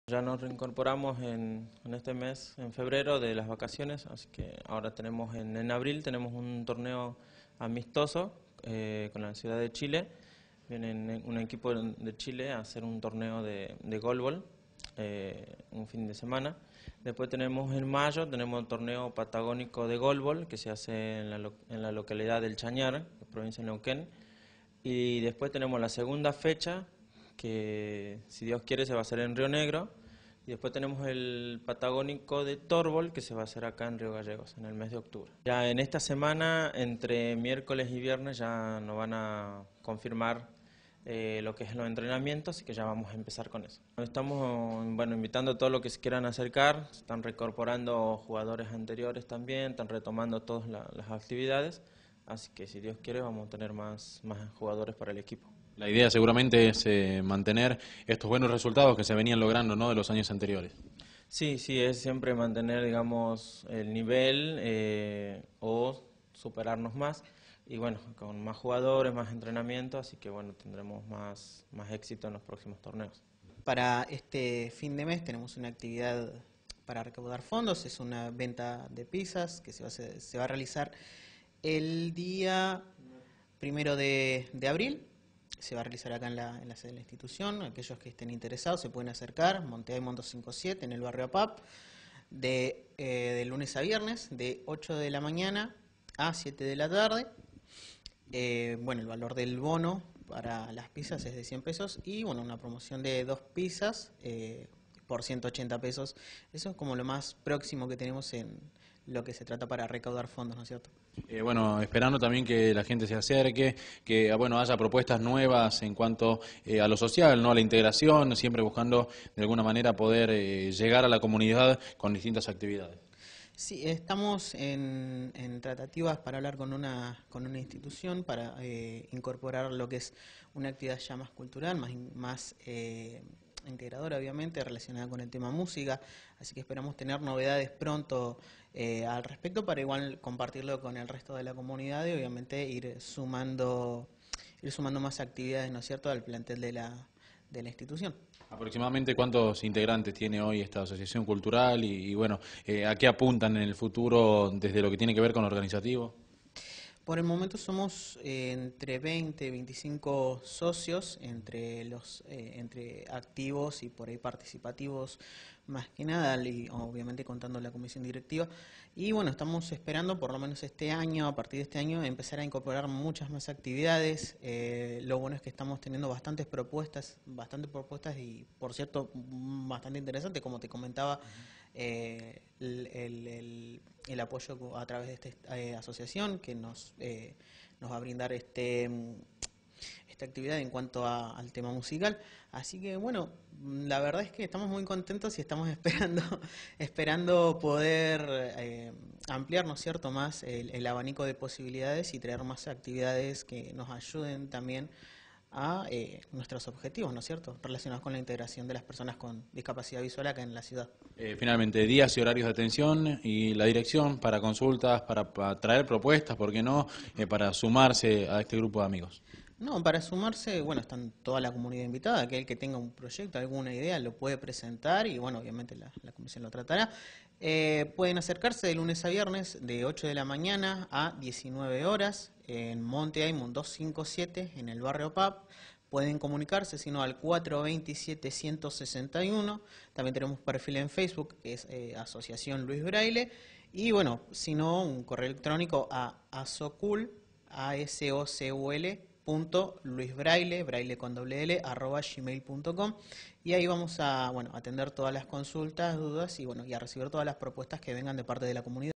ALEJANDRO TEJERINA- EZEQUIEL CÁRCAMO- COMENZARON LAS ACTIVIDADES ANUALES DE LA ASOCIACIÓN
VISITAMOS LA ASOCIACIÓN CULTURAL LUIS BRAILLE, LUGAR EN EL QUE COMENZARON LAS ACTIVIDADES ANUALES, ENTRE LAS QUE HAY ENTRE OTRAS, ACTIVIDADES DEPORTIVAS.
SUS REFERENTES ALEJANDRO TEJERINA Y EZEQUIEL CÁRCAMO INFORMARON SOBRE LAS ACTIVIDADES PARA RECAUDAR FONDOS, POR EJEMPLO UNA VENTA DE PIZZAS Y DISTINTAS INICIATIVAS PARA LOS VECINOS DE LA CIUDAD.